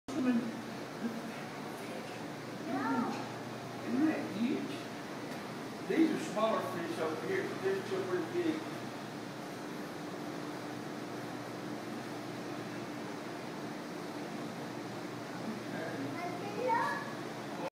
Papa Gator y HD
Viendo peces. Semana Santa 2012.